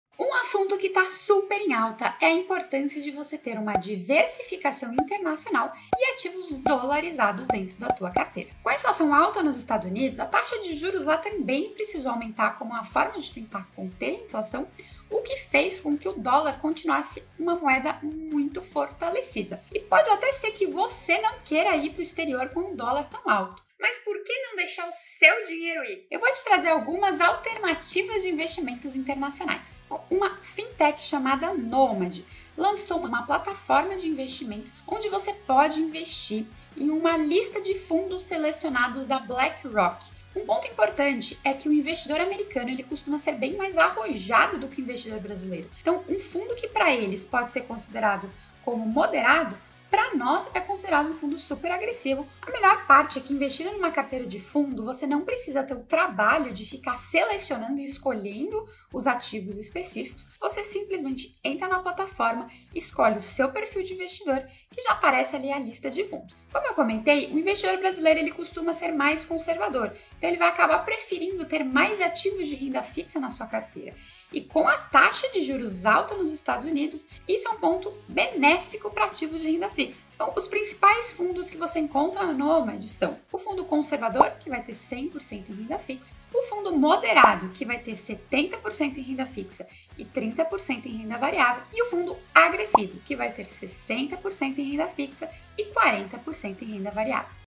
Você precisa diversificar a sua carteira de investimentos!
Assista o vídeo até o final e entenda a importância de você ter uma diversificação e ativos dolarizados na sua carteira.

Achou interessante?

💰💰💰Bons investimentos! 💰💰💰